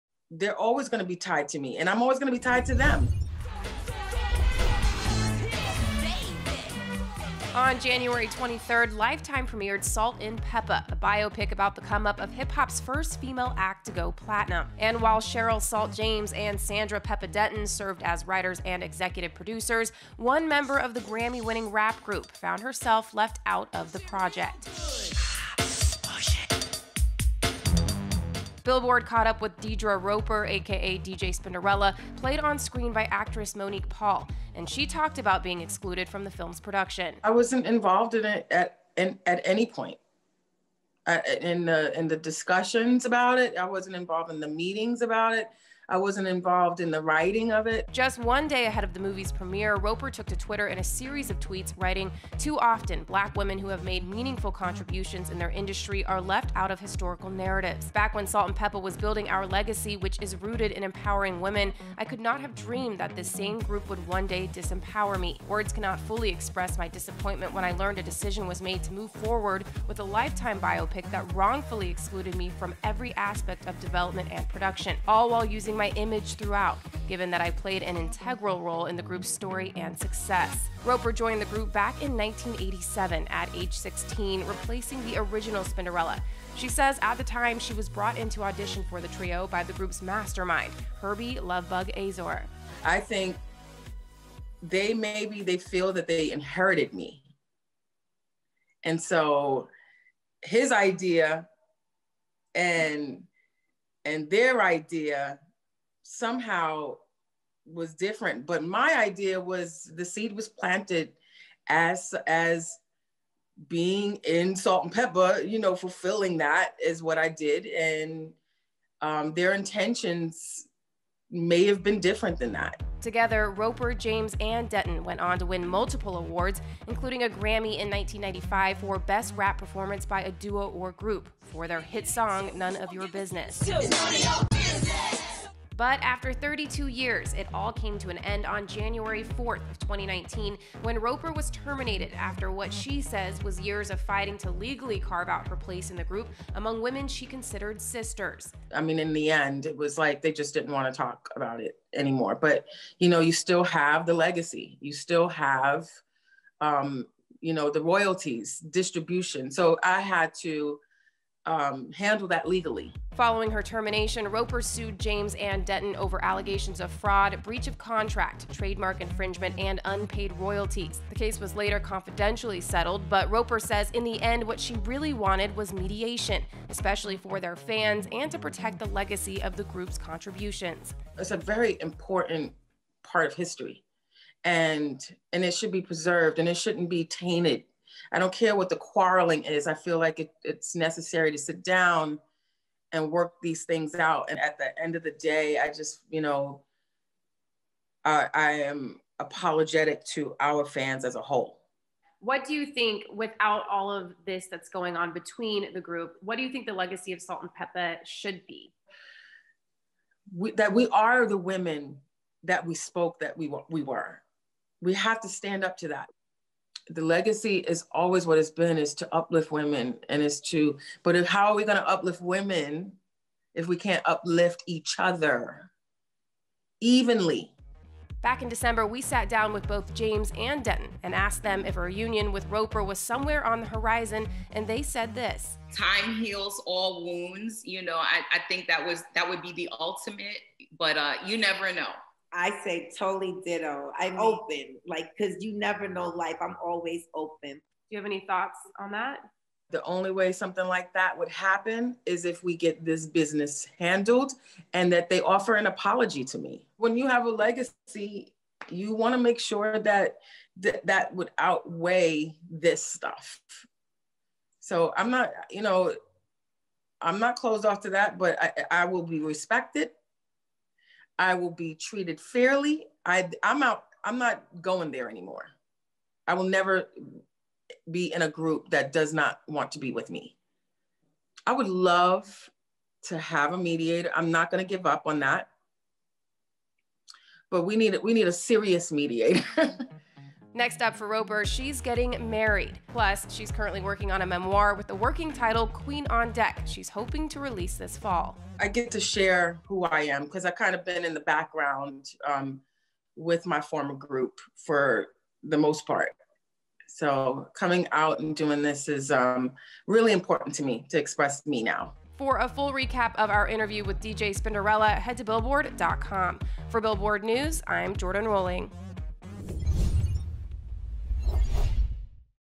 Deidra "DJ Spinderella" Roper Reveals What It Would Take For a Salt-N-Pepa Reunion | Billboard News
Billboard caught up with Deidra "DJ Spinderella" Roper following the premiere of Lifetime's 'Salt-N-Pepa' biopic and she reveals that she was left out of the project and had no involvement. Plus, she talks about what it will take to reunite hip-hop's first female act.